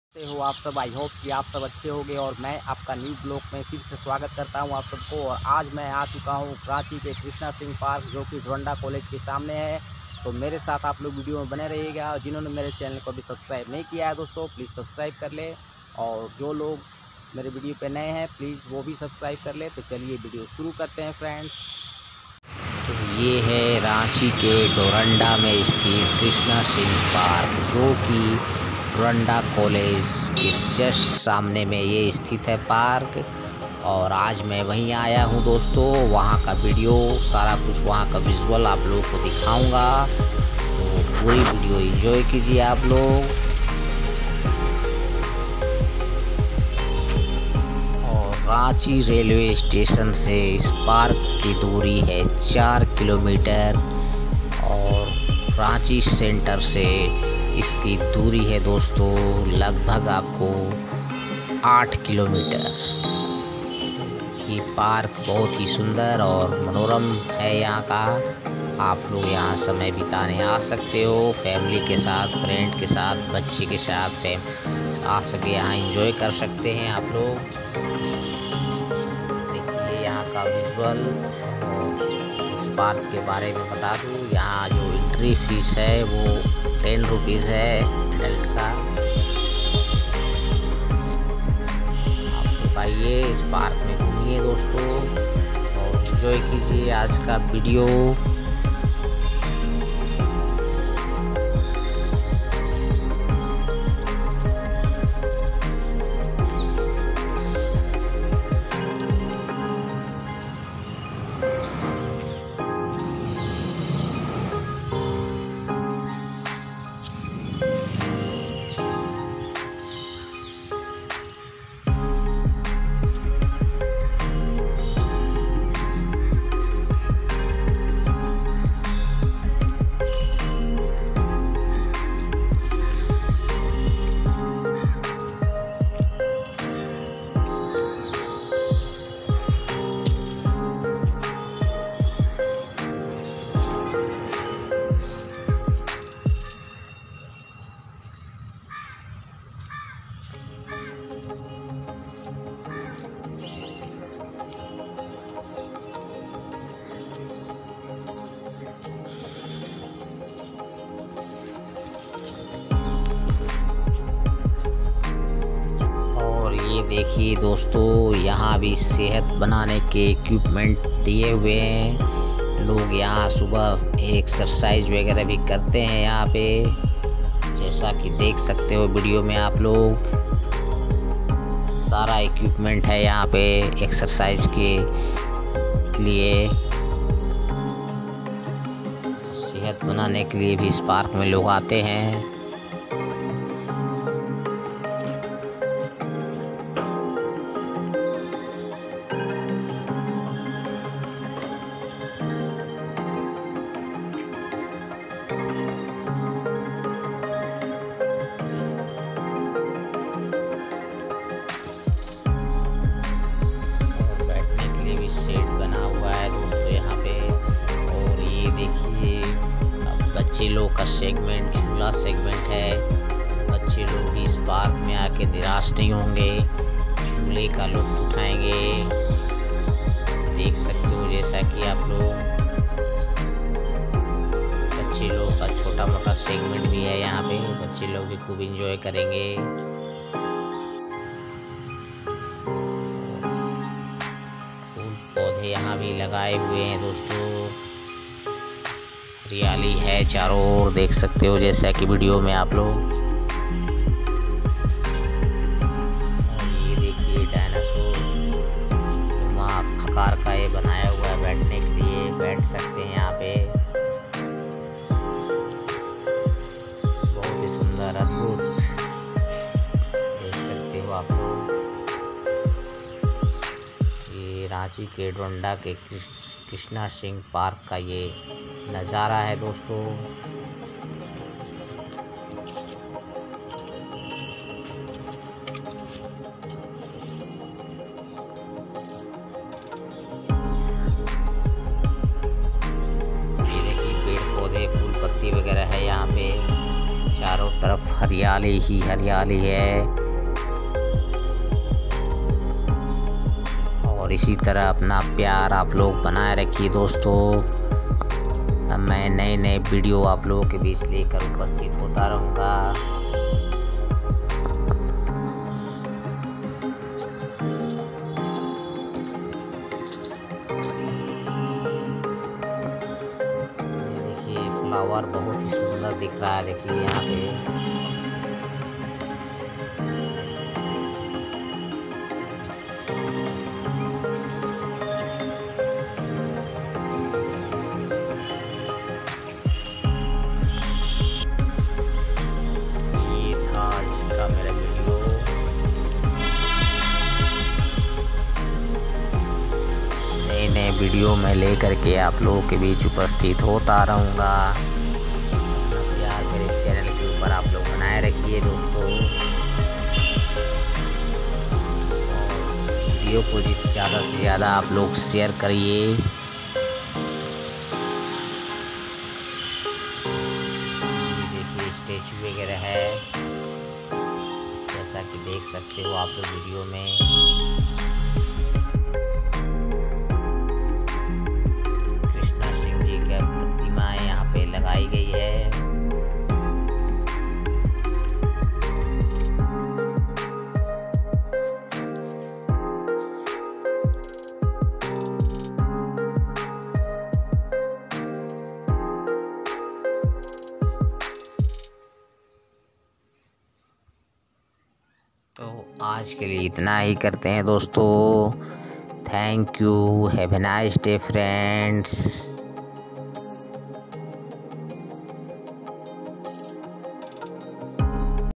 Ranchi Ka Krishna Singh park Doranda Ranchi.
रांची का कृष्णा सिंह पार्क जोकि डोरंडा में स्थित है डोरंडा कॉलेज के सामने यहां आने के लिए ऑटो बस अपनी प्राइवेट निजी वाहन से आ सकते हैं या रांची रेलवे स्टेशन से इसकी दूरी 4 किलोमीटर है और रांची सेंटर से इसकी दूरी 8 किलोमीटर है अपार बहुत ही सुंदर है और आकर्षक का केंद्र है झूला वगैरह बच्चे लोगों का है एक्सरसाइज के लिए इक्विपमेंट लगाए हुए हैं इस पार्क का एंट्री शुल्क ₹10 है।

mishra  8013.

keep watching.
keep sharing
Anil Mishra  82 official.